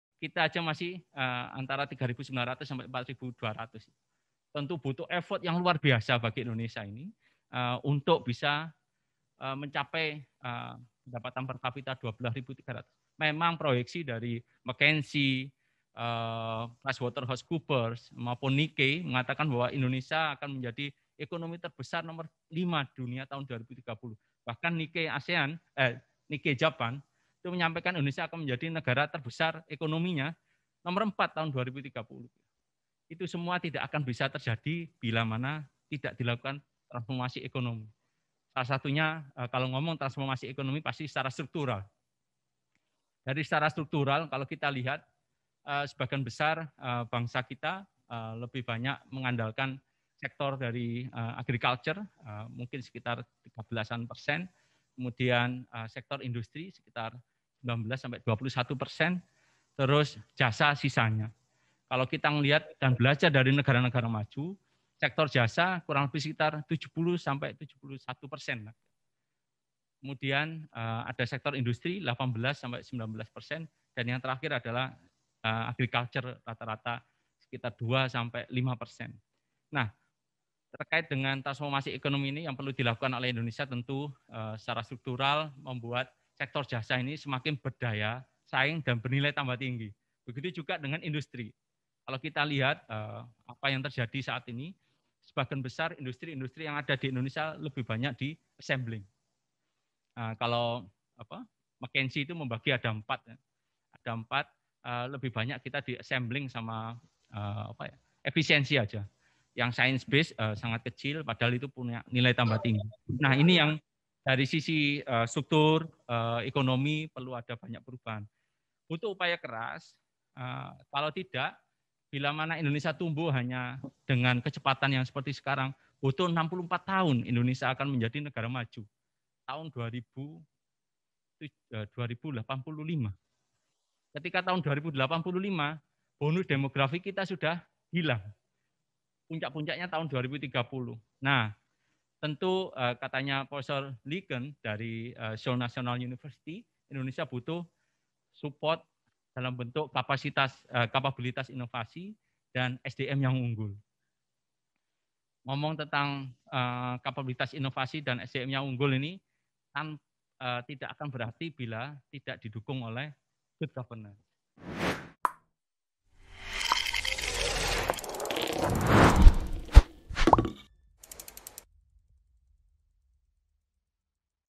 DIRECTOR'S REMARKS : CENTER OF GOOD GOVERNANCE AND BUREAUCRACY REFORM
DIRECTOR'S REMARKS
"Indonesia akan menjadi ekonomi terbesar nomor 5 ditahun 2030, Itu semua tidak akan terjadi jika tidak ada transformasi ekonomi dalam bangsa kita"